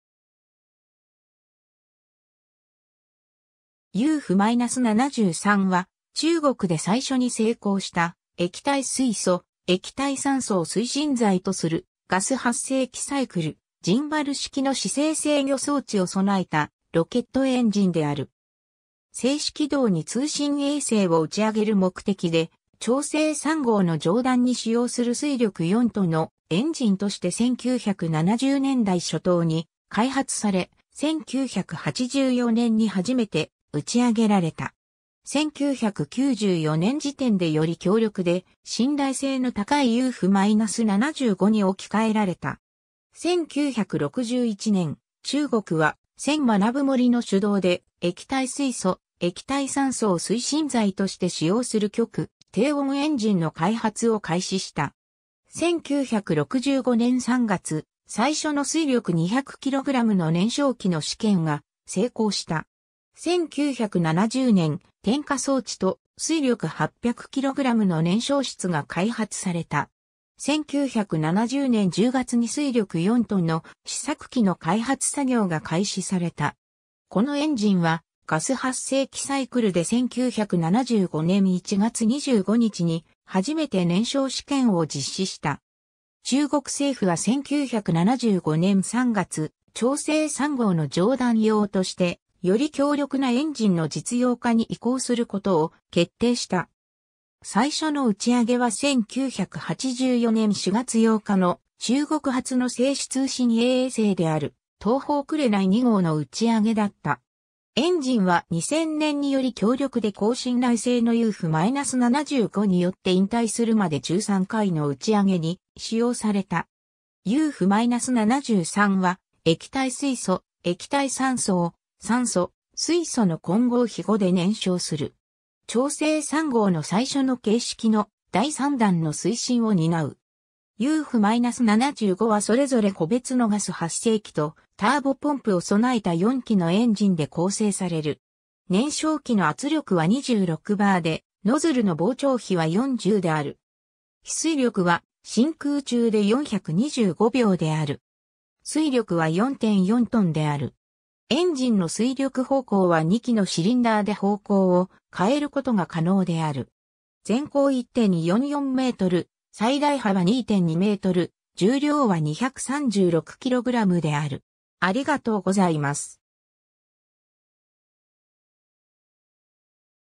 YF-73
液体水素/液体酸素を推進剤とするロケットエンジン
YF-73は、中国で最初に成功した液体水素/液体酸素を推進剤とするガス発生器サイクル、ジンバル式の姿勢制御装置を備えたロケットエンジンである。
 静止軌道に通信衛星を打ち上げる目的で長征3号の上段に使用する推力4トンのエンジンとして1970年代初頭に開発され、1984年に初めて打ち上げられた。
1994年時点でより強力で信頼性の高いYF-75に置き換えられた。
 1961年、中国は銭学森の主導で液体水素/液体酸素を推進剤として使用する極低温エンジンの開発を開始した。
1965年3月、最初の推力200kgの燃焼器の試験が成功した。
1970年、点火装置と推力800kgの燃焼室が開発された。
1970年10月に推力4トンの試作機の開発作業が開始された。
このエンジンはガス発生器サイクルで1975年1月25日に初めて燃焼試験を実施した。
中国政府は1975年3月、長征3号の上段用として、より強力なエンジンの実用化（311計画）に移行することを決定した。
最初の打ち上げは1984年4月8日の中国初の静止通信衛星である東方紅2号の打ち上げだった。
エンジンは2000年により強力で高信頼性のYF-75によって引退するまで13回（そのうち3回は失敗）の打ち上げに使用された。
 YF-73は液体水素/液体酸素を酸素/水素の混合比5で燃焼する。
長征3号の最初の型式の第3段の推進を担う。
YF-75はそれぞれ個別のガス発生器とターボポンプを備えた4基のエンジンで構成される。
燃焼器の圧力は26 barでノズルの膨張比は40である。
比推力は真空中で425秒である。
推力は4.4トンである。
エンジンの推力方向は2基のシリンダーで方向を変えることが可能である。
全高1.44m、最大幅2.2m、重量は236kgである。